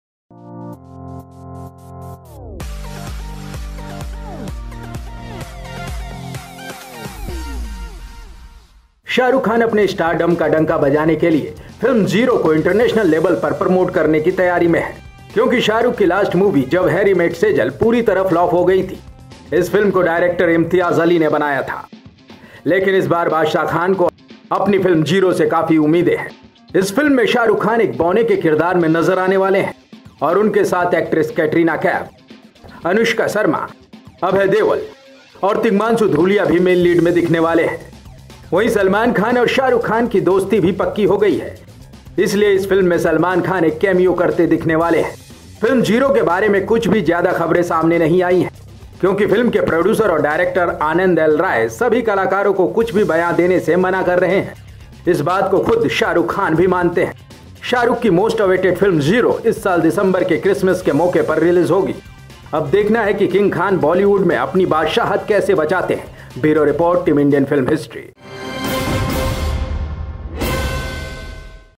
Shahrukh's Special International Plan To Promote His Film ZERO | IFH
Address:
1/3,Shere Punjab Colony,
Andheri East.
Mumbai 400 093
India.